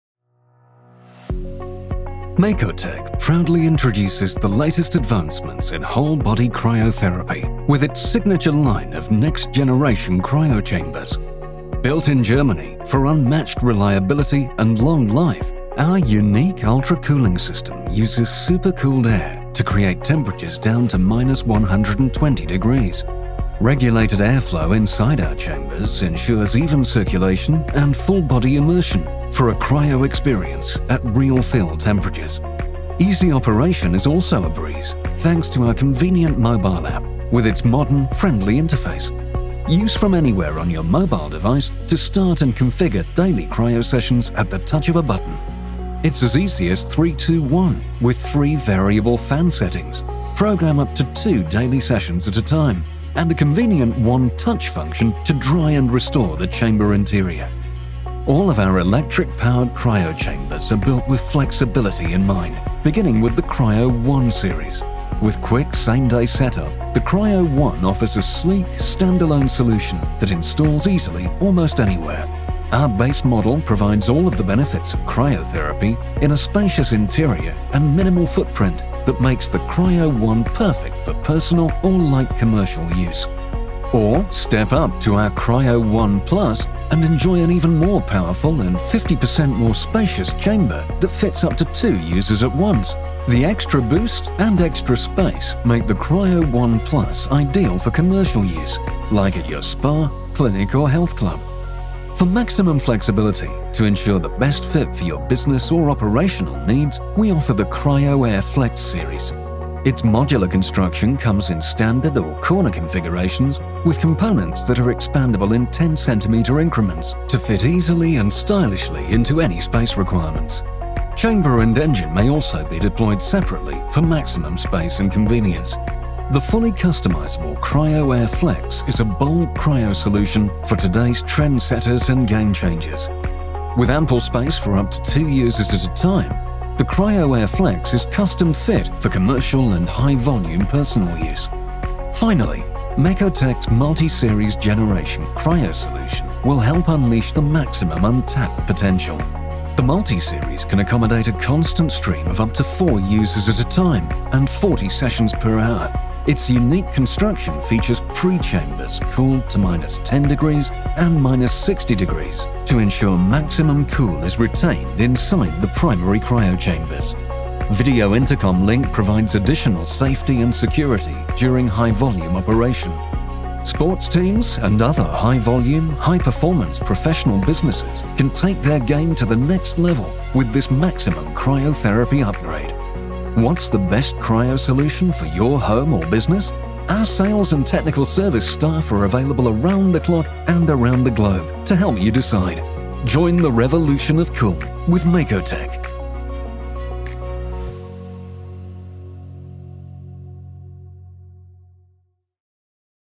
Cryo chambers powered by MECOTEC
Cross over into the next generation of performance support and enhanced
well-being with the latest breakthrough in whole-body cryotherapy. MECOTEC brings the power of cryotherapy into your home or business, with its innovative line of certified, German-engineered cryotherapy chambers.